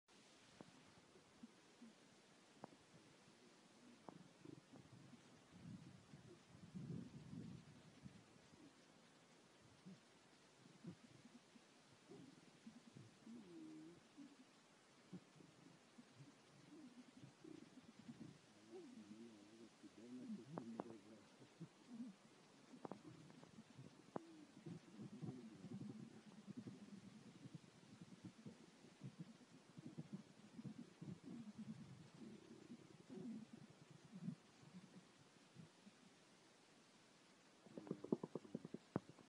Pareja de Gorilas, 고릴라 커플, Пара горилл, زوجان الغوريلا., 大猩猩夫妇, Càraid Gorilla.
Gorilas, wallwildredespaña.
Pareja de Gorilas. غوريلا, Горилла, 大猩猩, ゴリラ, Con khỉ đột, गोरिल्ला, Gorila.
El gorila de montaña ( Gorilla beringei beringei ) es una de las dos subespecies del gorila oriental. Está catalogado como en peligro por la UICN a partir de 2018.

Hay dos poblaciones: una se encuentra en las montañas volcánicas Virunga de África Central , dentro de tres Parques Nacionales : Mgahinga , en el suroeste de Uganda ; Volcanes , en el noroeste de Ruanda ; y Virunga , en el este de la República Democrática del Congo (RDC). La otra población se encuentra en el Parque Nacional Impenetrable de Bwindi en Uganda . Algunos primatólogos especulan que la población de Bwindi es una subespecie separada, aunque no se ha finalizado ninguna descripción. En junio de 2018, había más de 1,000 personas.